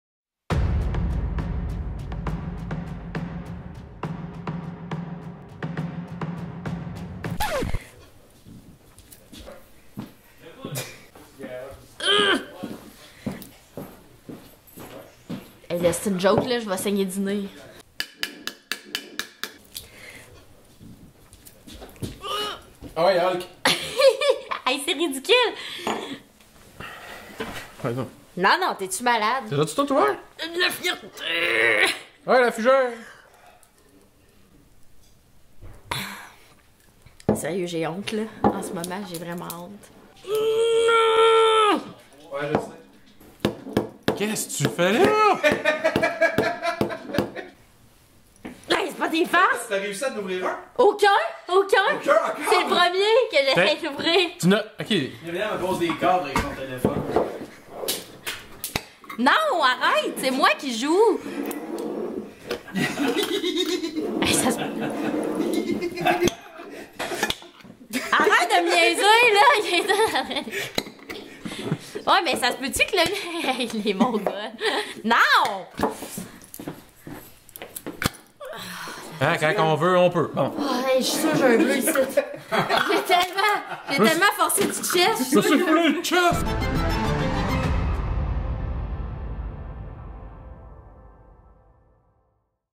Salsa Teaser - S01E11x
Parfois, on laisse tourner les caméras entre deux capsules!!! Myriam Fugère est extrêmement divertissante même quand elle n’est pas en représentation. 😂🤡